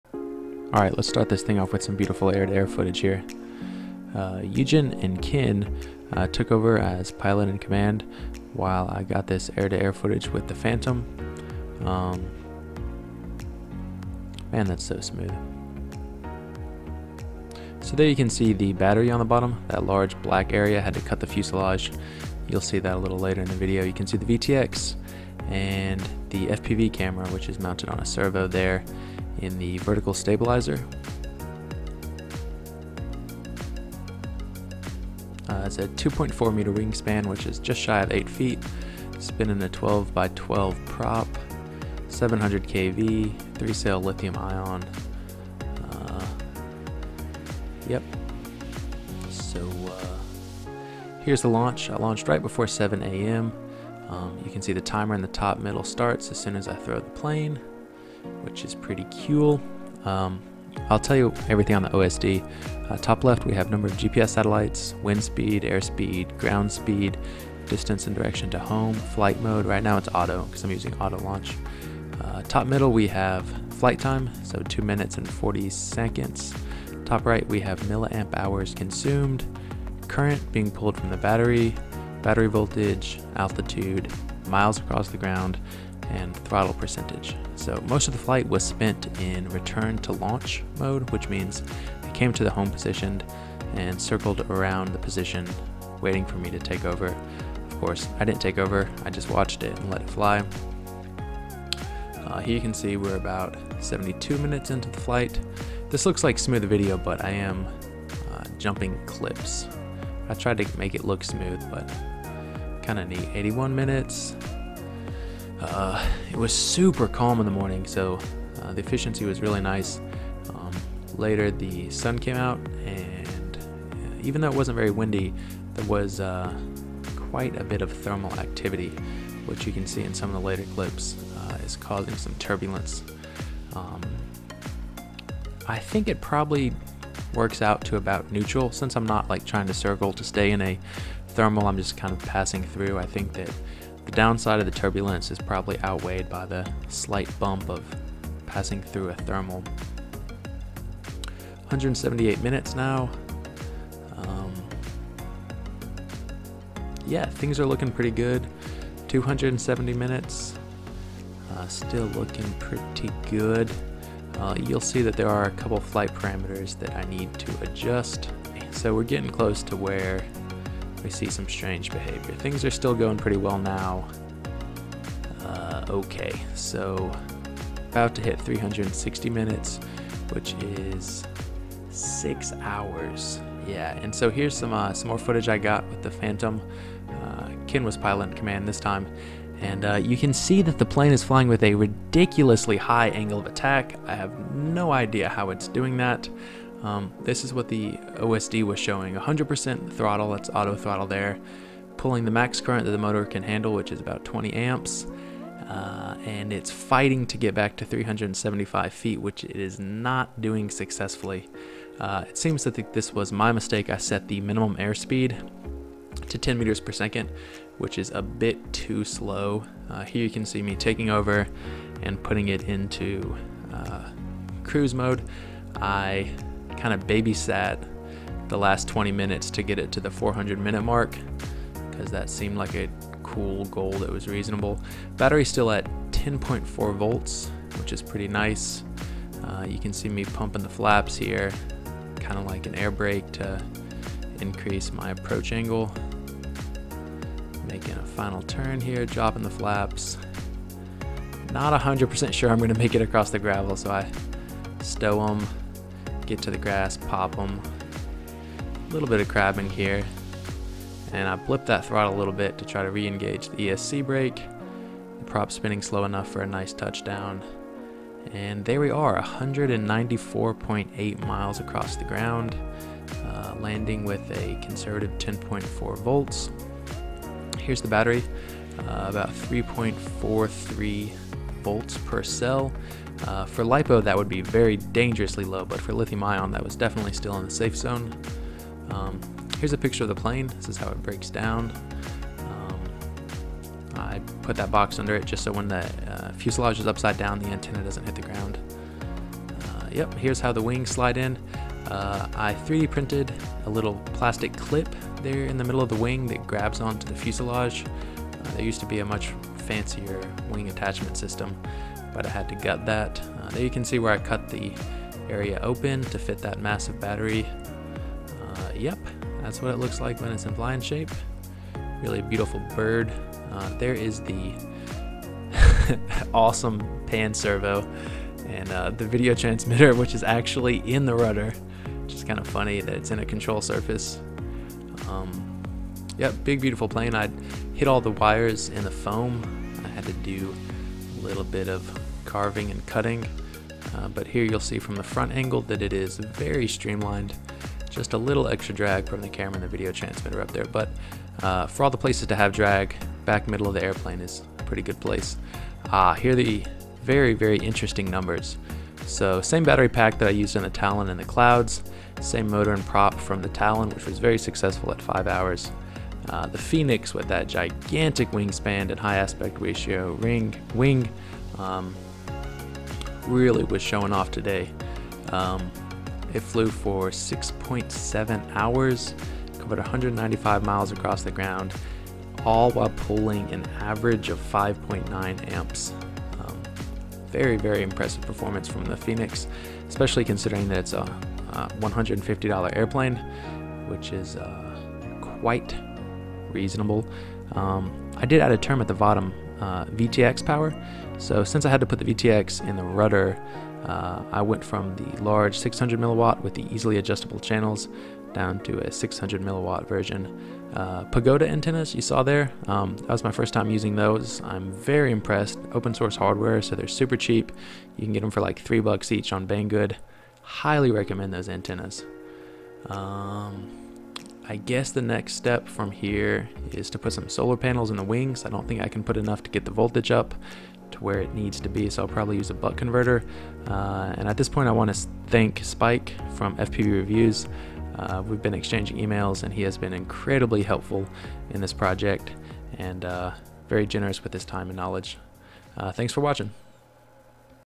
Phoenix 2400 First Autonomous Endurance Flight
This was my new record for time and distance (401 minutes and 195 miles)
Phoenix 2400
Matek F405 Wing with Arduplane
12x12 APC propeller
Racerstar 3508 700KV motor
DYS 30A ESC with BLheli
45Ah 3s Li-Ion battery that I made with Samsung 50E cells